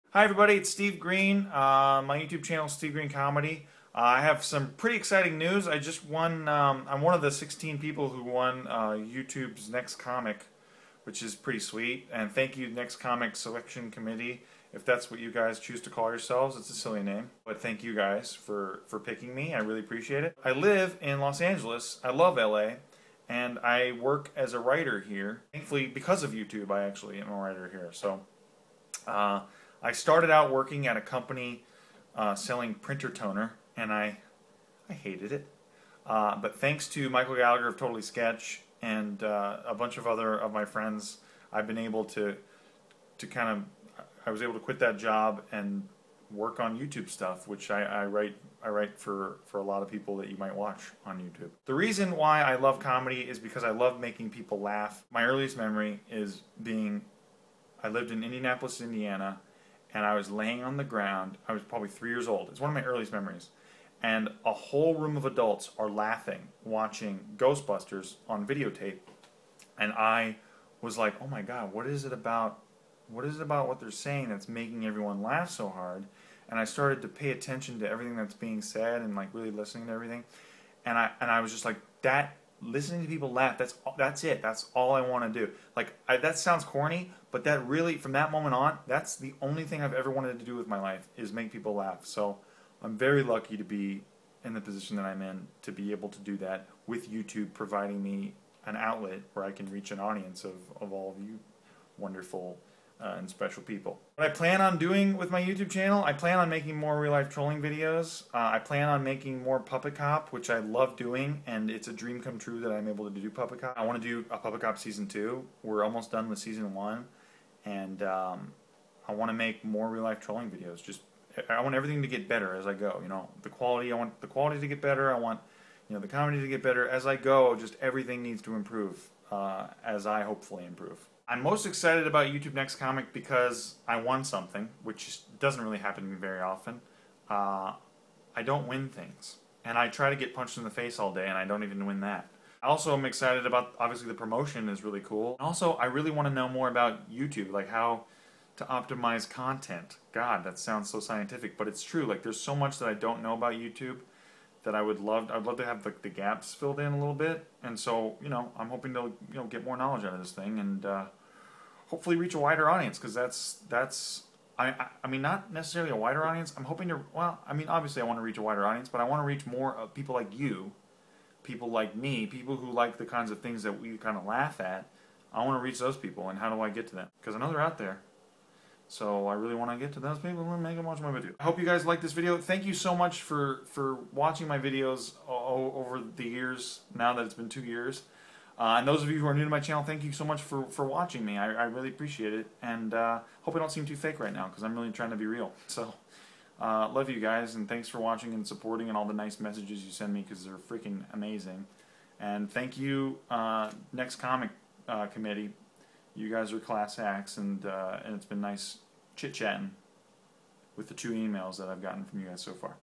Thank You!!
SteveGreeneComedy is genuine & sincere for one video only. I was selected for the Youtube Next Comic competition thanks to all of YOU guys!!